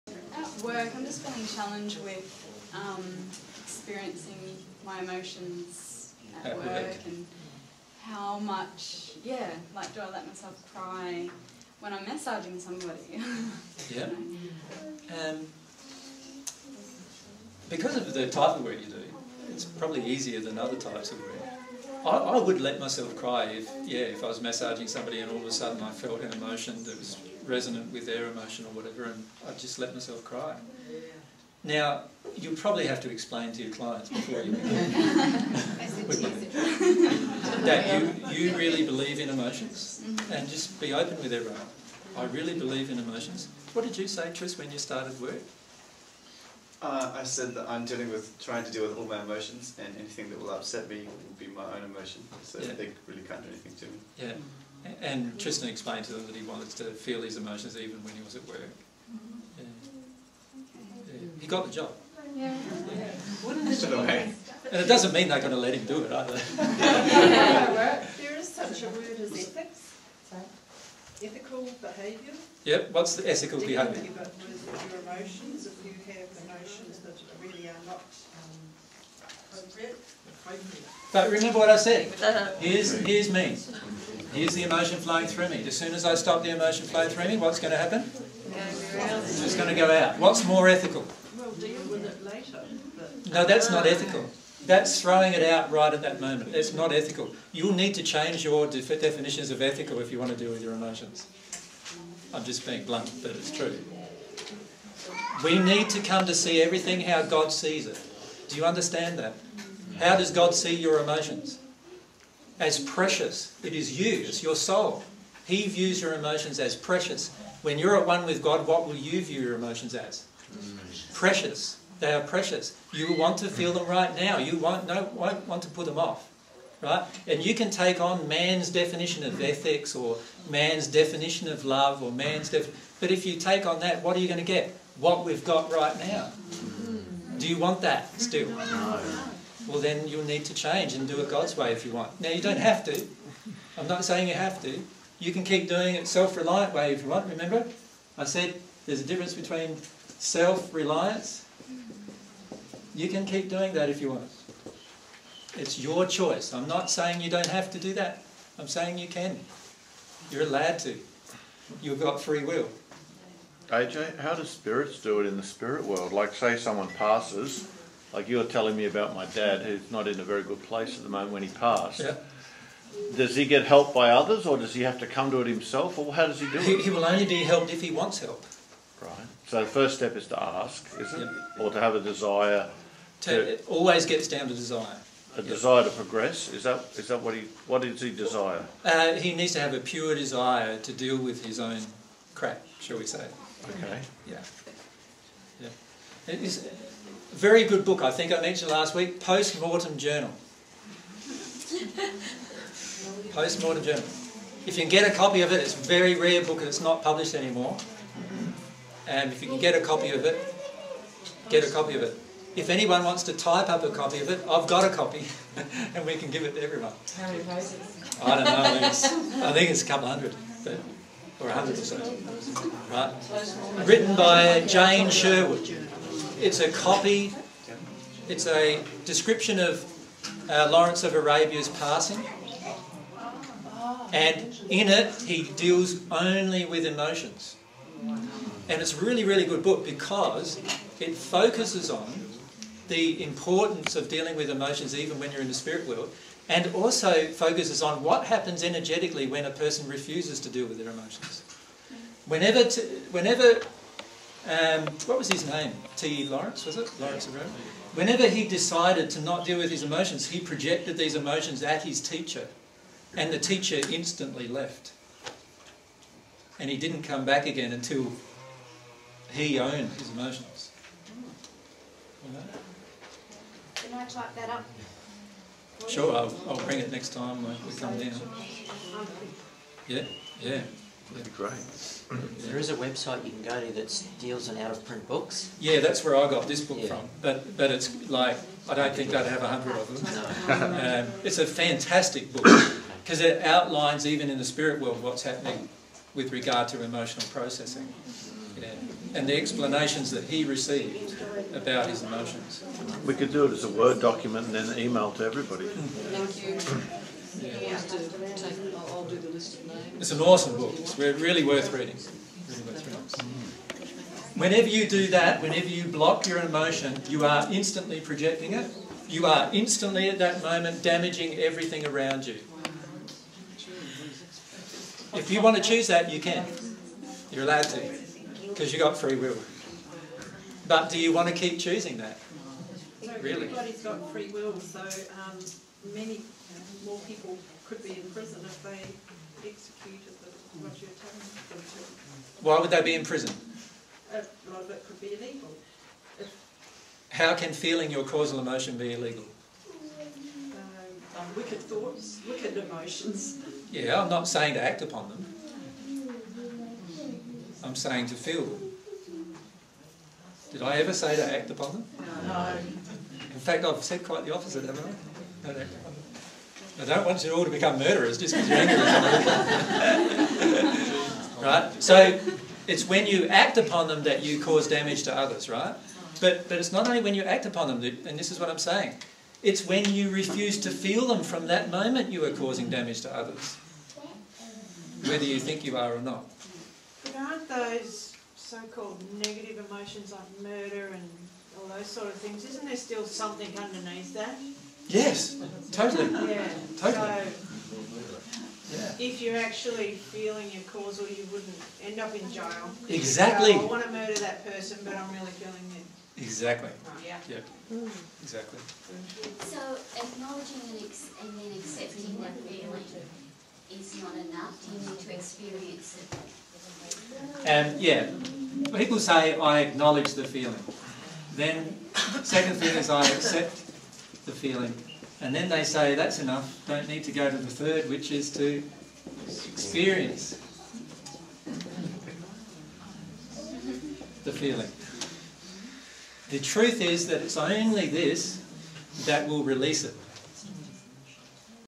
Jesus on Ethics and Feeling Emotions at Work
Jesus explains how choosing to feel all of our emotions is the ethical choice from God's perspective.

dtstart:022603:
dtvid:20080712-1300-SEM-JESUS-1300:
dtmd5:cbe5a7fe26dc004e237a4e5b6acfda1e: